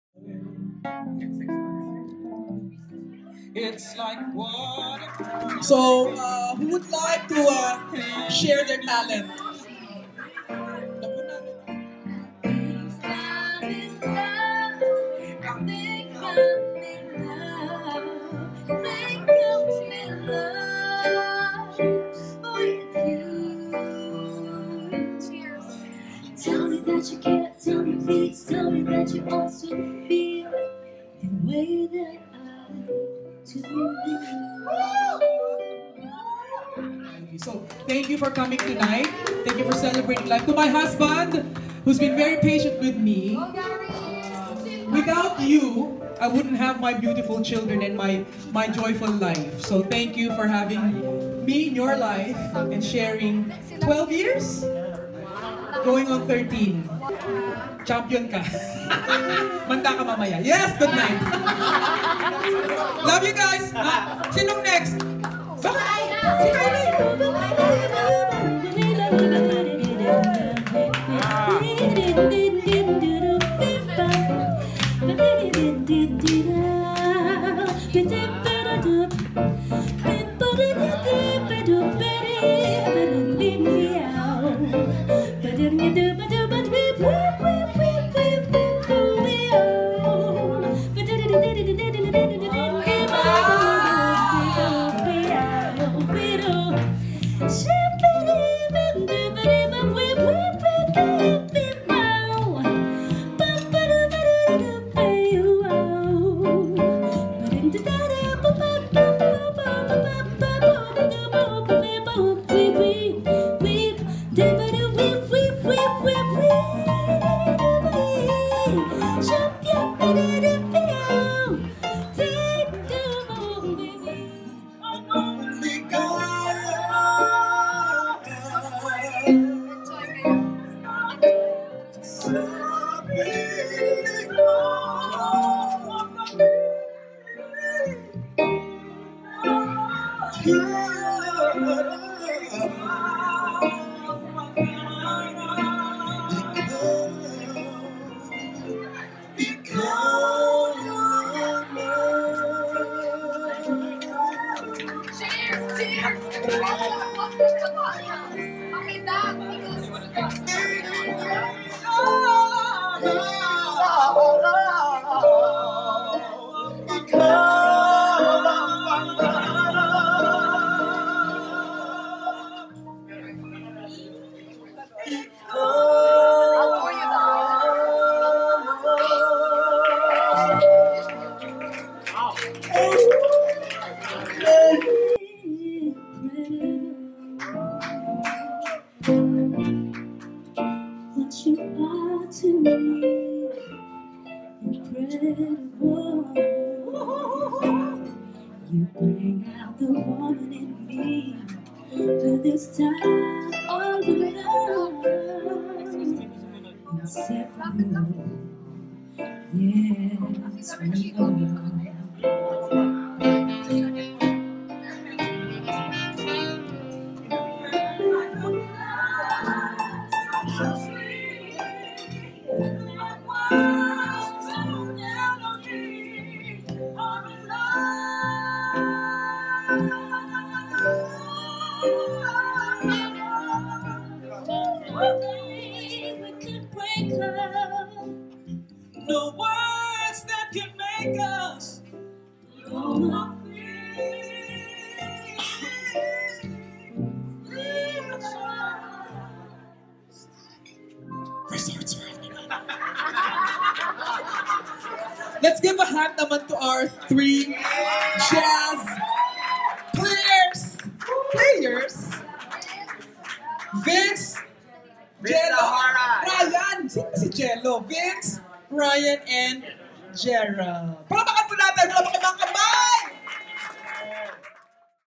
Jaya's Birthday
A very intimate Birthday Celebration/Jam with top talents and executives of the industry.